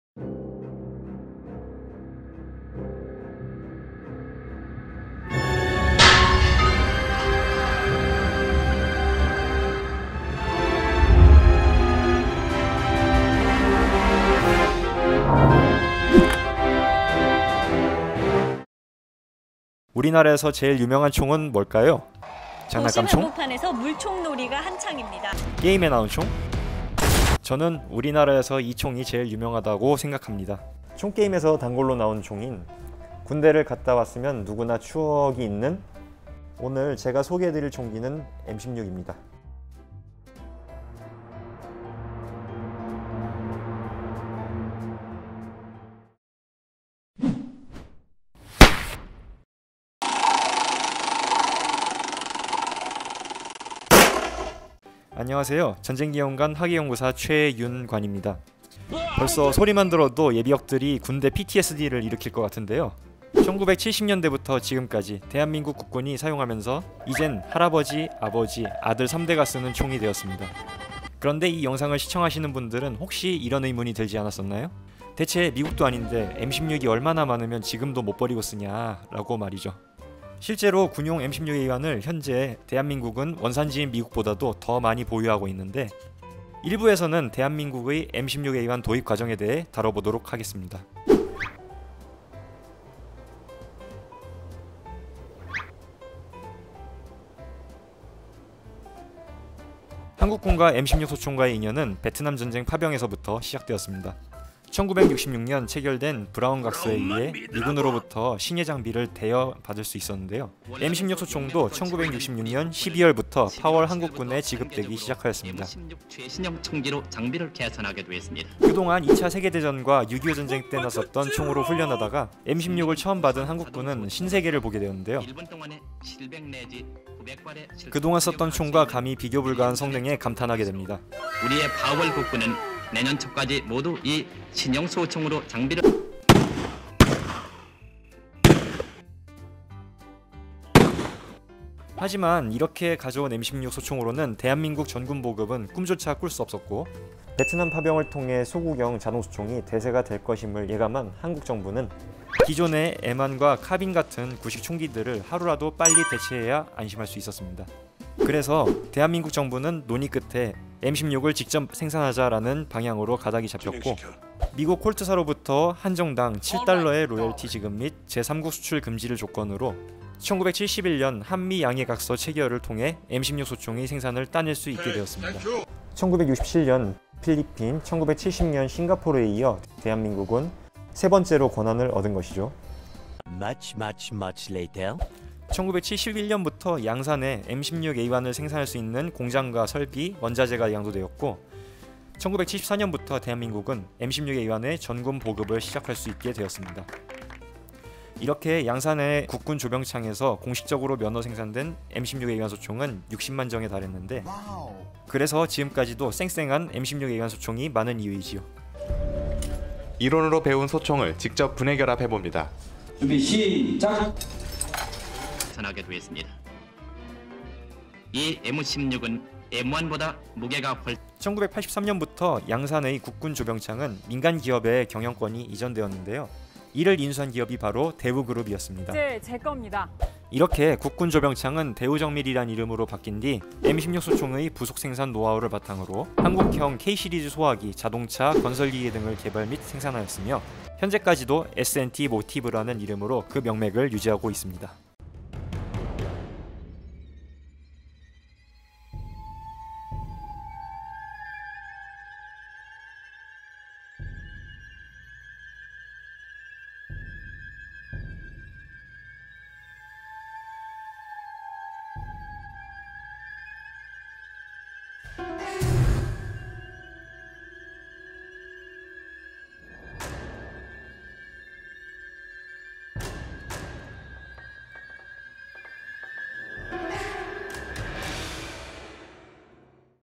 [밀덕×학예사] 대한민국 예비군의 영원한(?) 친구! 대한민국 M16A1 소총 도입사
M16A1 1편은 대한민국의 국방을 현재까지 책임지고 있는 미국의 M16A1 소총의 대한민국 국군 도입까지의 역사를 담고 있습니다.